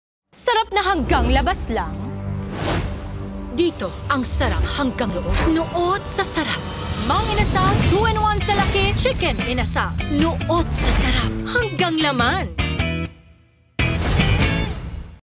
Mang Inasal Chicken Inasal "Crisostomo" 15sec (Philippines, 2019)
With Dimples Romana.

Nuot sa Sarap!.

P99 Lang One Rice

Price Varies.